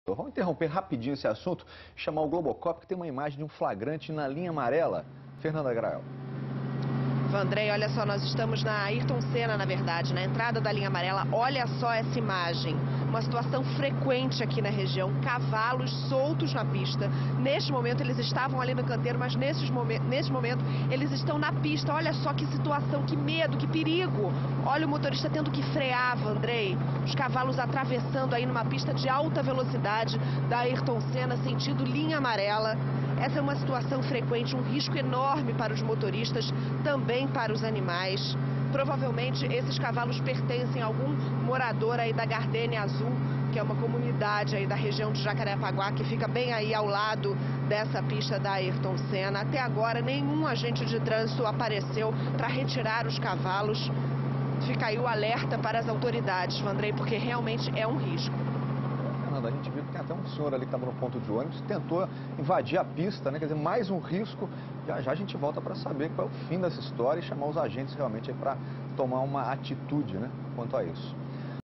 Cavalos circulam em pistas da Avenida Ayrton Senna
Sexta-feira, 05/08/2011
Cavalos são flagrados na Avenida Ayrton Senna na manhã de sexta-feira (5). Os animais circulam livremente e atrapalham o fluxo dos carros. A presença dos cavalos aumenta o risco de acidentes na região.